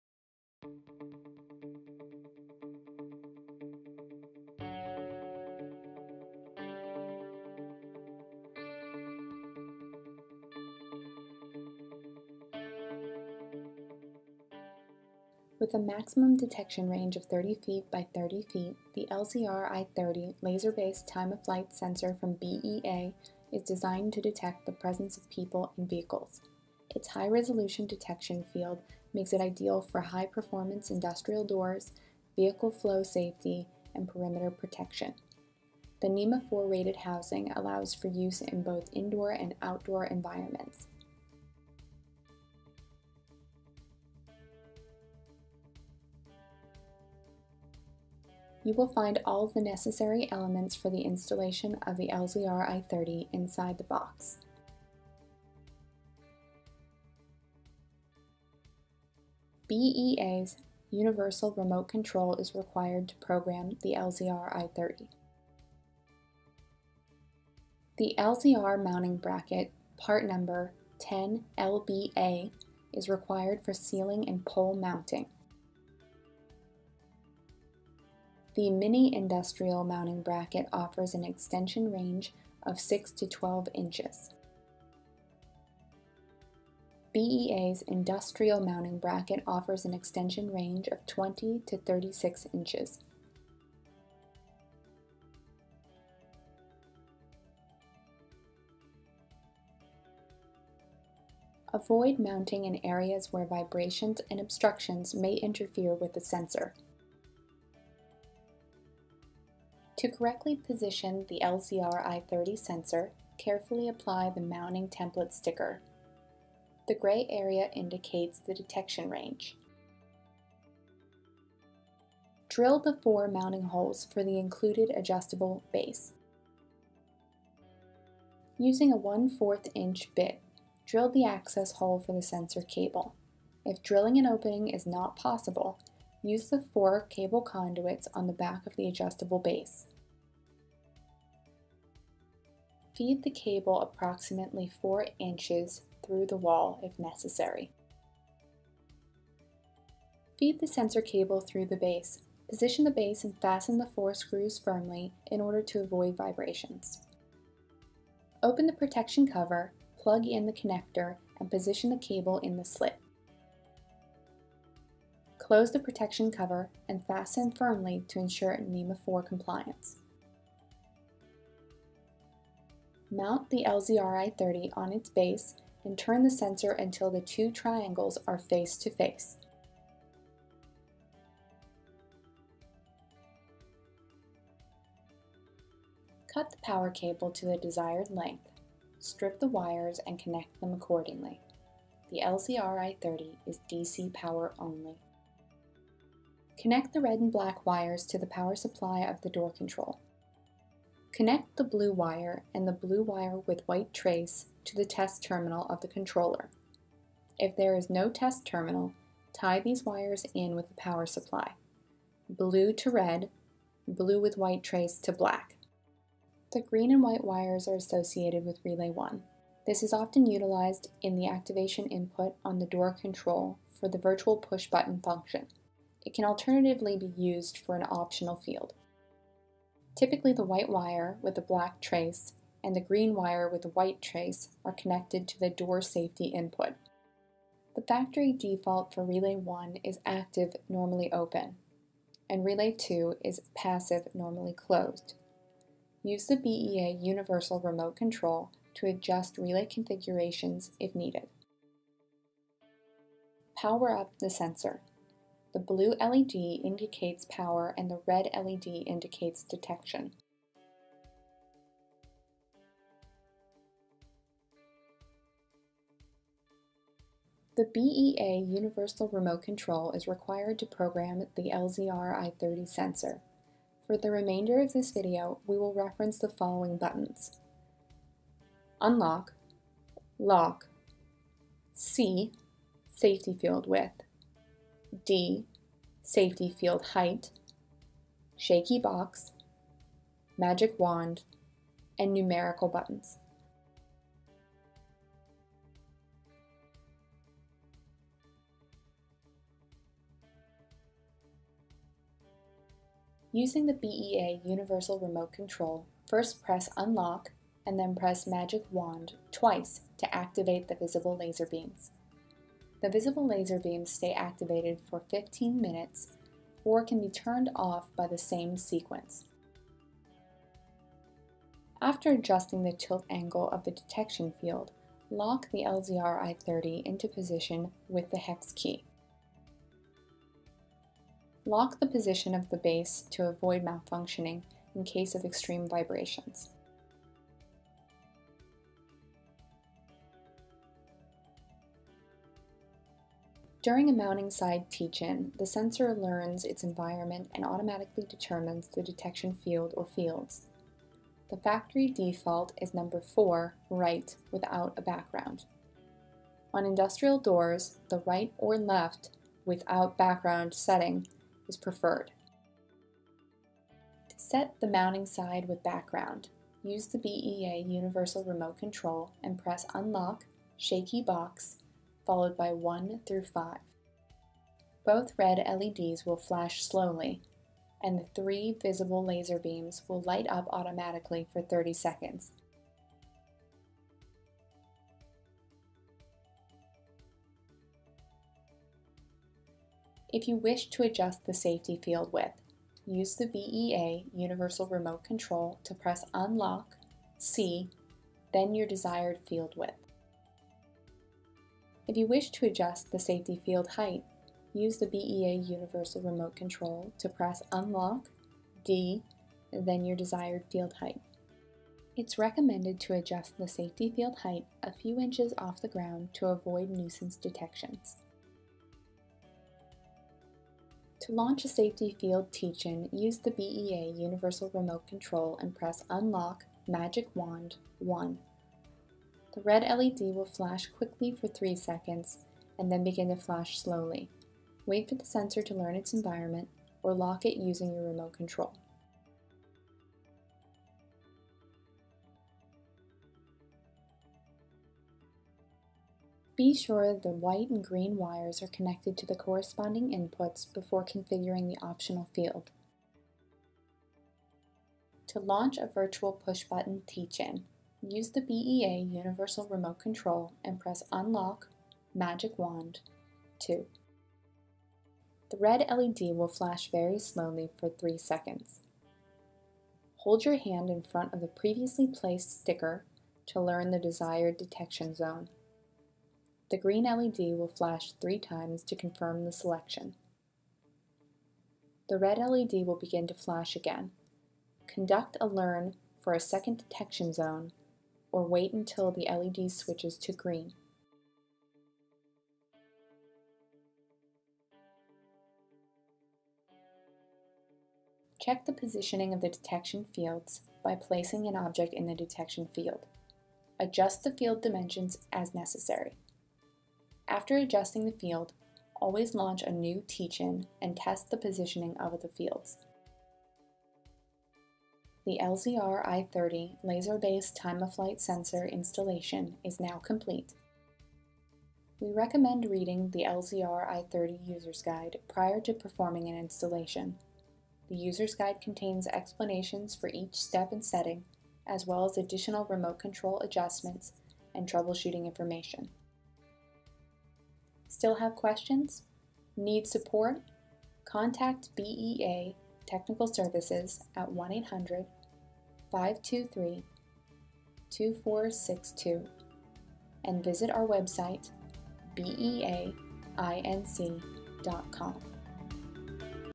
Cách lắp đặt cảm biến cửa tự động BEA LZR I30 How to Install BEA LZR I30
PGTech nhà phân phối Cảm biến cửa tự động hay còn gọi là cảm biến an toàn cửa tự động của thương hiệu cảm biến cửa tự động xuất xứ Châu Âu, Hãng BEA của Bỉ là thương hiệu cảm biến cửa tự động lớn nhất thế giới.

Video sau hướng dẫn cách lắp đặt cảm biến cửa tự động Model LZR I30 là dòng cảm biến cửa tự động công nghiệp dùng để  nhận diện chuyển động của các loại xe như xe tải, xe nâng...sử dụng để đi lại qua cửa của các nhà kho, xưởng, nhà máy, kho hàng...bằng công nghệ Laser.

Sử dụng cảm biến an toàn cửa tự động Model BEA LZR I30 giúp nhận diện hiệu quả, an toàn cao
Hiện này PGTech là nhà phân phối chính thức các dòng cảm biến an toàn cửa tự động của thương hiệu BEA (Bỉ), đại diện hãng tại Việt Nam